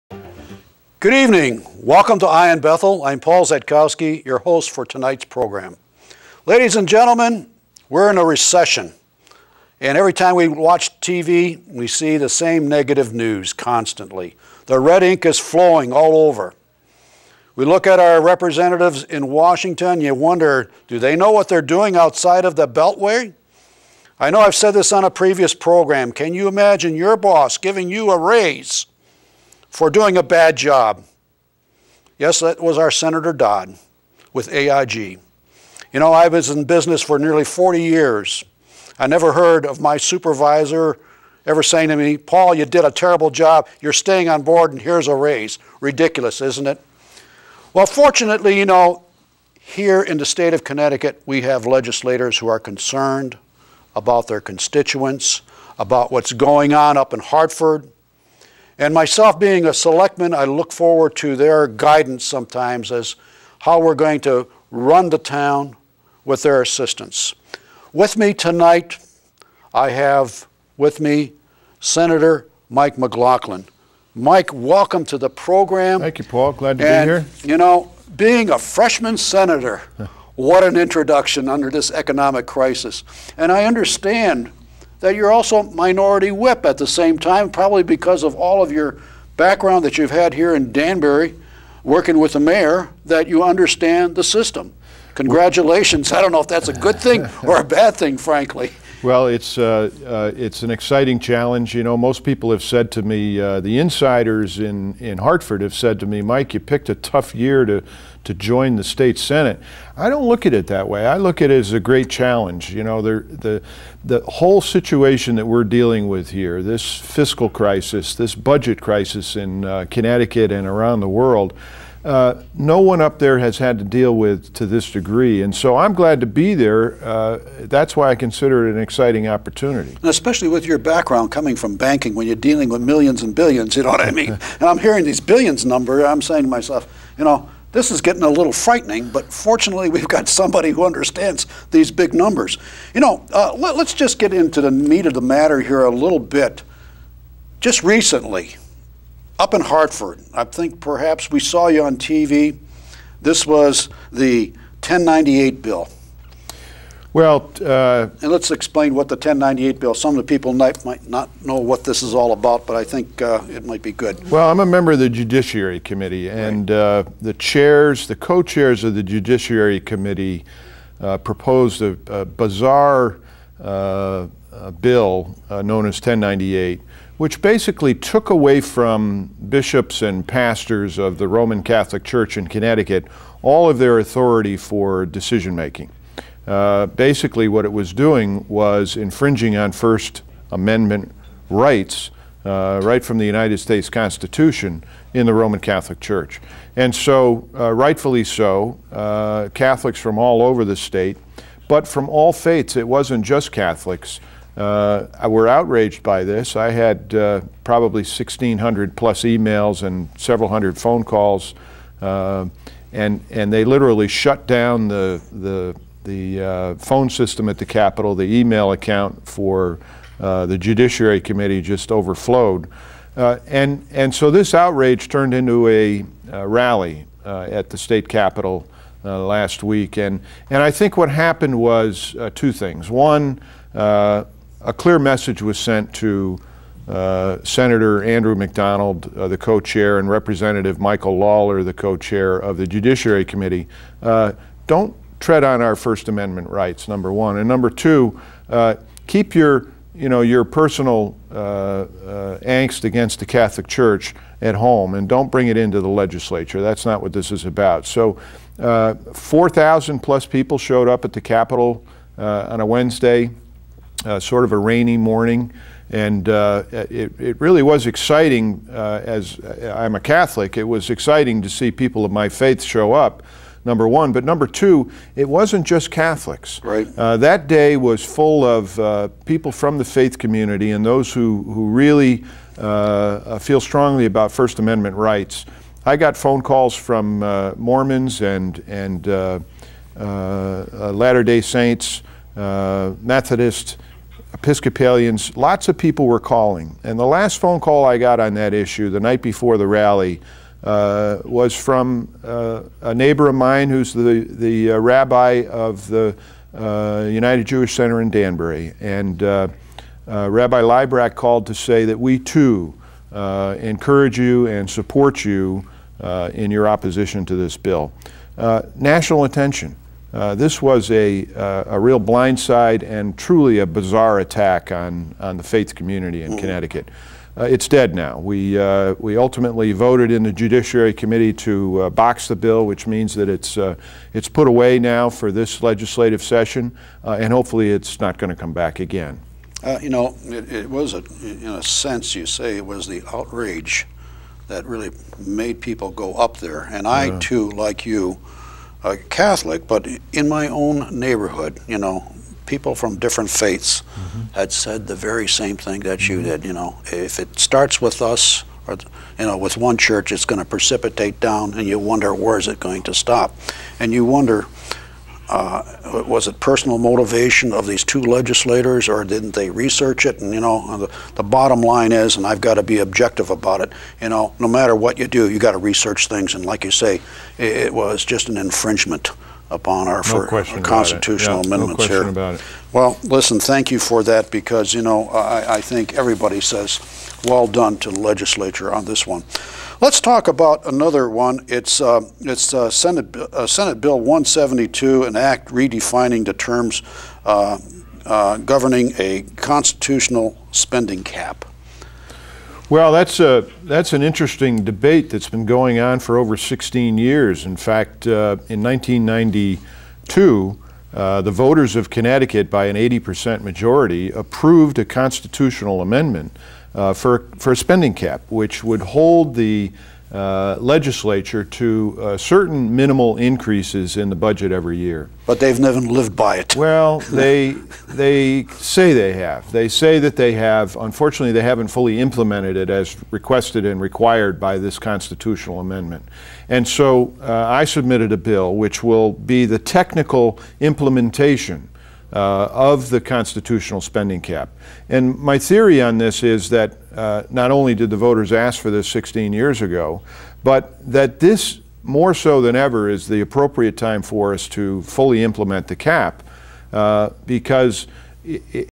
Michael McLachlan 001
Eye on Bethel presents State Senator Michael McLachlan, District 24.

The discussion will be hosted by Paul Szatkowski.

Issues to be discussed are Connecticut's State Budget, revenue sharing with the communities as well as educational funding.